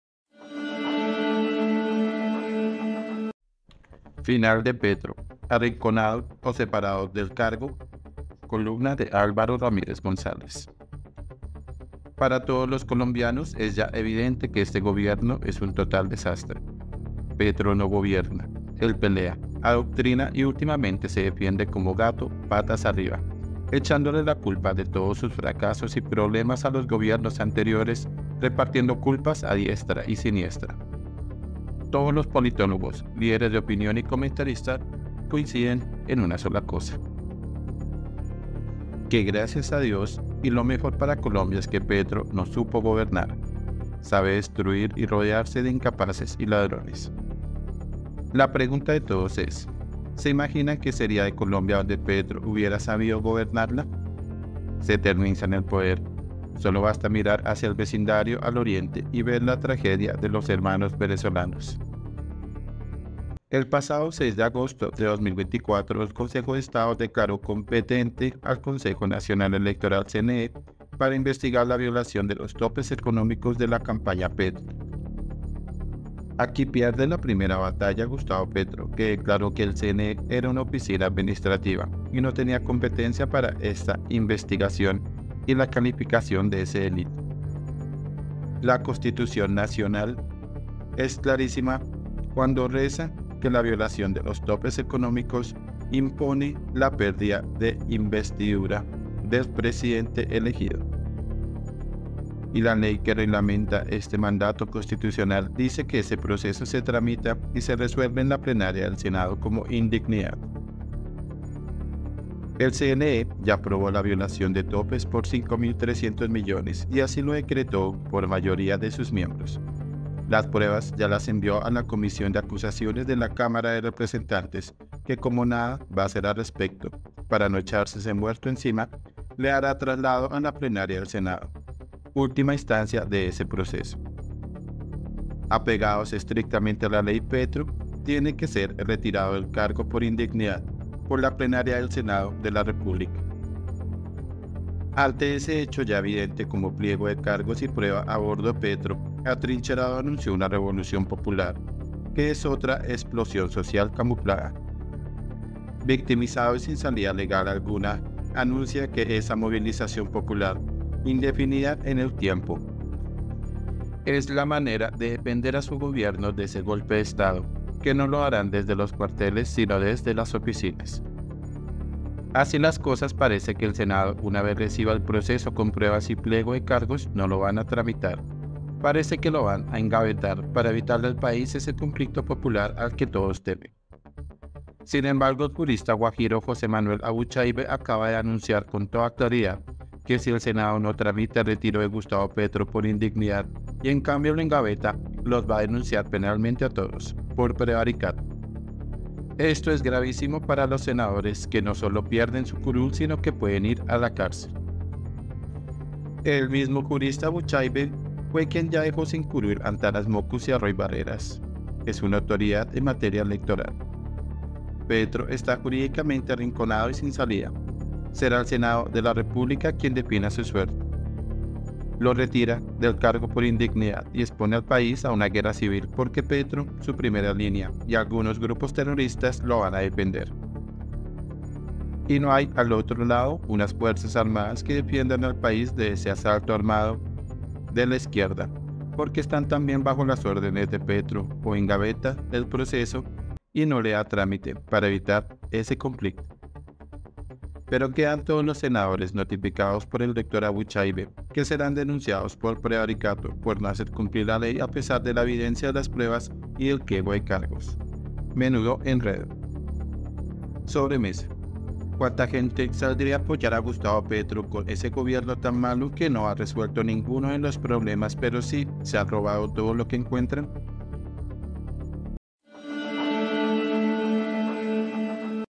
¿FINAL DE PETRO: ARRINCONADO O SEPARADO DEL CARGO? 🔴 Por: Álvaro Ramírez González
Para todos los colombianos es ya evidente que este gobierno es un total desastre, Petro no gobierna èl pelea, adoctrina y últimamente se defiende como gato patas arriba; echándole la culpa de todos sus fracasos y problemas, a los gobiernos anteriores repartiendo culpas a diestra y siniestra.